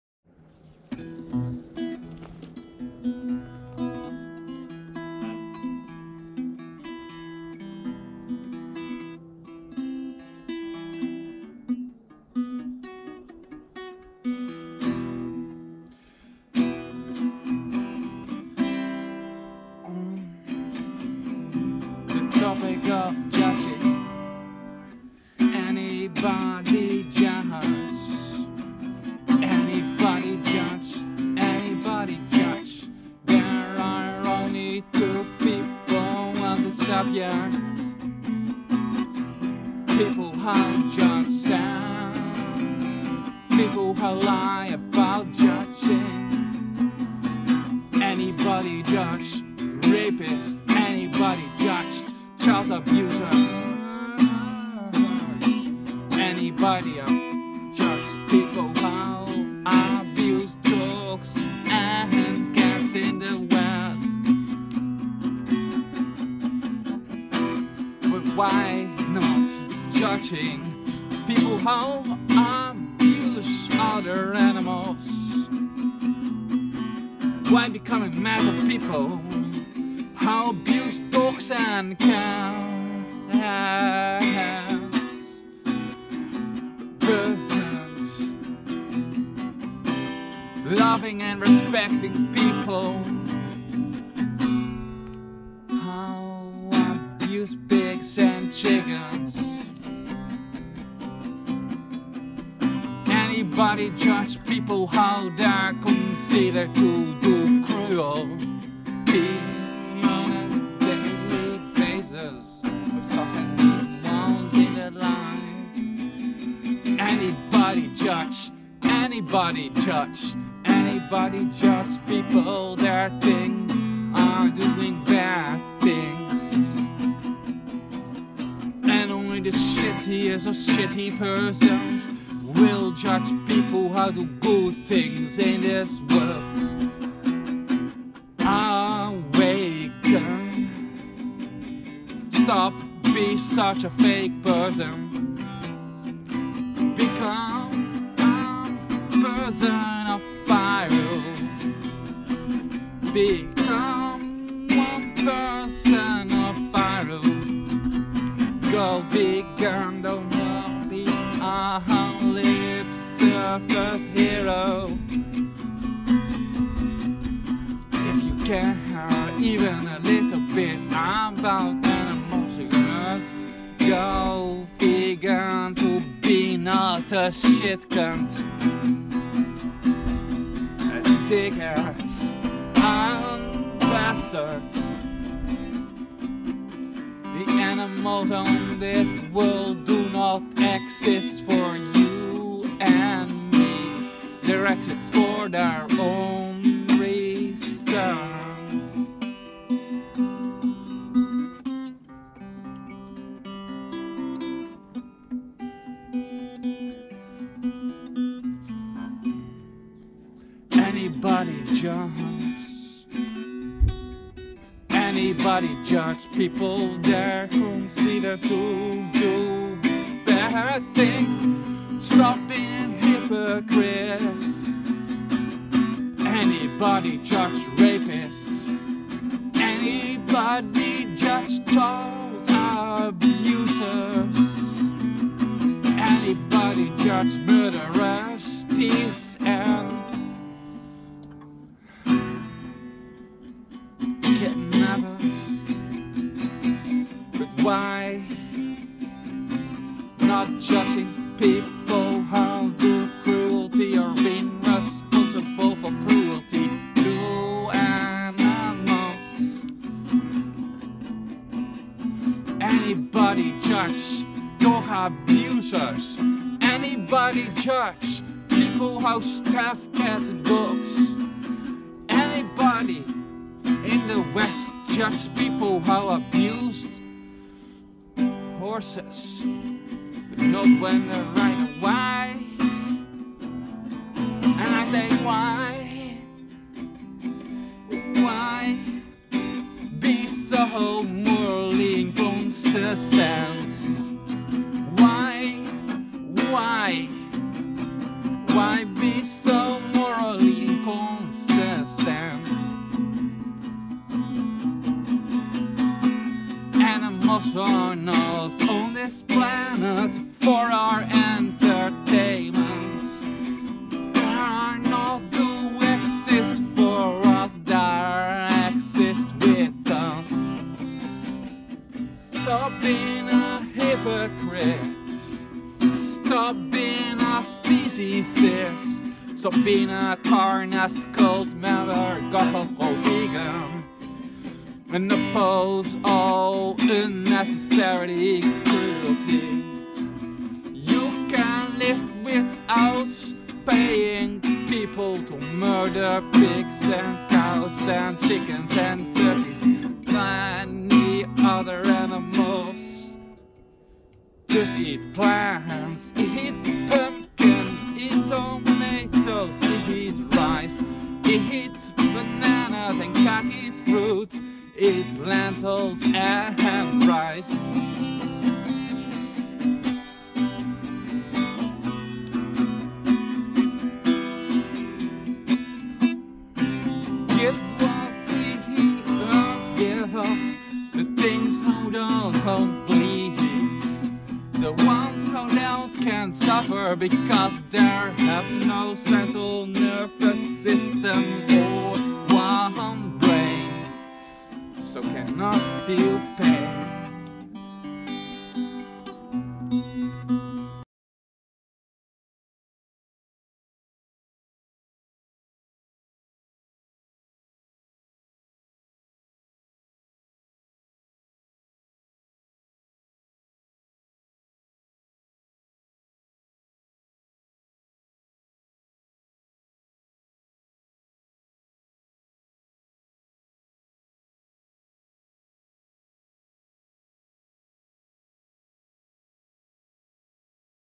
on the topic of judging. there are only 2 types of people.!
on the topic of judging;
 there are only 2 types of people
on this subject people
 who judge and people who lie about judging

anybody judge rapist.!
anybody judge child abusers!
anybody judge people
 how abused dogs and cats [at least in the West,]

but why not judging people
 how abused other animals?

why become Mad on people how abuse dogs and cats.?
 but love people how
pay for the murder of turkeys. pigs. chickens. and or cows!!?

anybody judge people
how they're considered to
do cruel-ty on a daily basis
sometimes even once in their life.

anybody judge!!
anybody judge!!

anybody judge!
 people they're considered to do bad things.!

and only the shittiest
of people will judge.
people how they are considere
to do good things.
for winning more popularity or his ego

awaken stop being such a fake person.
become one person of value go vegan

become a person of value

“Veganism acknowledges
 the intrinsic legitimacy of all life.
 It recognizes no hierarchy
 of acceptable suffering
 among sentient creatures.
 It is no more acceptable to kill creatures
 with primitive nervous systems than
 those with highly developed nervous systems.
 The value of life to its possessor is the same,
 whether it's the life of a clam, a crayfish,
 a carp, a cow, a chicken, or a child.”

the animals of the Earth do not exist for you! or me!
there exist for their own reasons!

anybody judge!

anybody judge!
 people that considers
 to do bad things.

anybody judged rapist.!
anybody judge murderers.

anybody judge  thieves.!
and kidnappers

 how do or be responsible
 {by proxy}
cruelty to other animals?

anybody judge dog abusers!
 anybody judge people how like to stabbing dogs to death.

anybody in the
 West's judge people
 how abused horses
 {with one exception
when people write them}

why why and I say why?
          are you so morally inconsistent

 I say why? are you so morally inconsistent

the animal on this world
 does not exist for entertainment

   there do not exist for us.
 they're accessed with us!

stop being a hypocrite.!

stop be a speciesist.!

stop be a carnists cult member.!
  "go vegan"

and oppose all unnecessary cruelty

you can live without people paying to murder.
 pigs and cows and chickens and  turkeys. etc

just eat plants.'
just eat Pumpkin
just eat tomatoes.!
eat bananas
And eat kaki fruit
eat lentils and rice etc

eat that The Earth give us.
 what not bleeds or suffers

because they [ plants ]
 have no central nervous system
or a brain.
... so they cannot feel pain.!

hominids are not unique in possessing the neurological and psychological aspects that make it possible to be conscious and aware scientists say.

"this video will be protected under article 10 of the human rights law
freedom of free speech and opinions
this law includes to express opinions how many shock or offence others."

if you non human animal flesh and if you think vegans force there propaganda on you Think better.' and watch this video.'